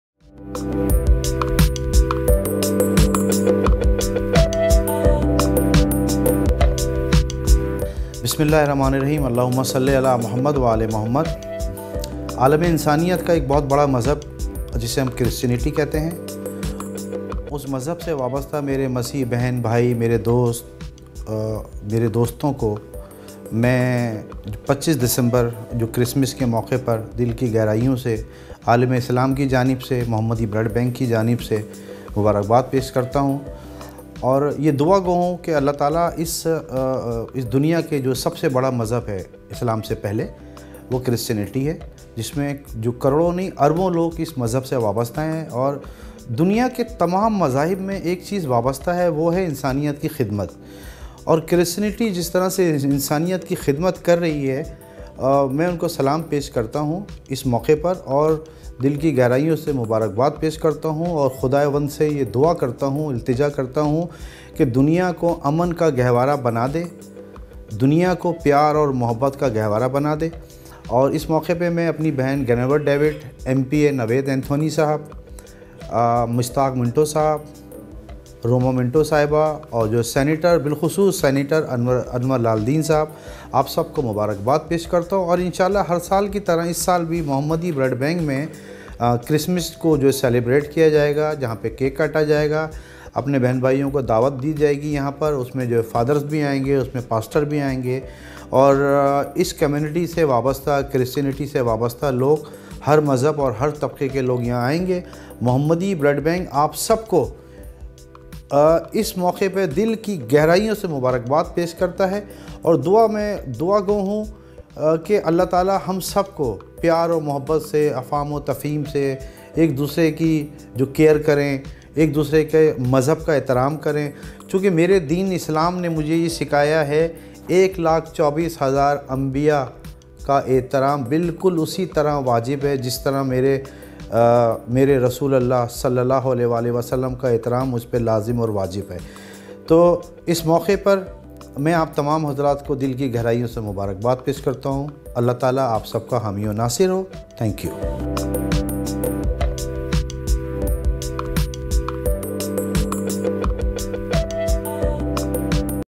CEO Muhammadi Foundation Message About Christmas...
محمدی بلڈ بینک کے چیف ایگزیکٹو آفیسر سید مہدی رضوی کا کرسمس کے موقع پر ویڈیو پیغام
محمدی بلڈ بینک کے چیف ایگزیکٹو آفیسر سید مہدی رضوی کی جانب سے مسیحی برادری کو کرسمس کی مبارکباد
دنیا کے تمام مذاہب میں ایک بات یکساں ہے اور وہ ہے انسانیت کی خدمت، سید مہدی رضوی
انسانیت کی خدمت میں مسیحی برادری نے ہمیشہ بڑھ چڑھ کر حصہ لیا ہے، سید مہدی رضوی
انسانیت کی خدمت میں دنیا بھر کی مسیحی برادری کے کردار کو ہمیشہ یاد رکھا جائے گا، سید مہدی رضوی
25دسمبر کو محمدی بلڈ بینک کی جانب سے کرسمس سے متعلق تقریب منعقد کی جائے گی، سید مہدی رضوی
کرسمس کی تقریب میں مسیحی برادری کی اہم شخصیات سمیت شہریوں کی بڑی تعداد شریک ہوگی، سید مہدی رضوی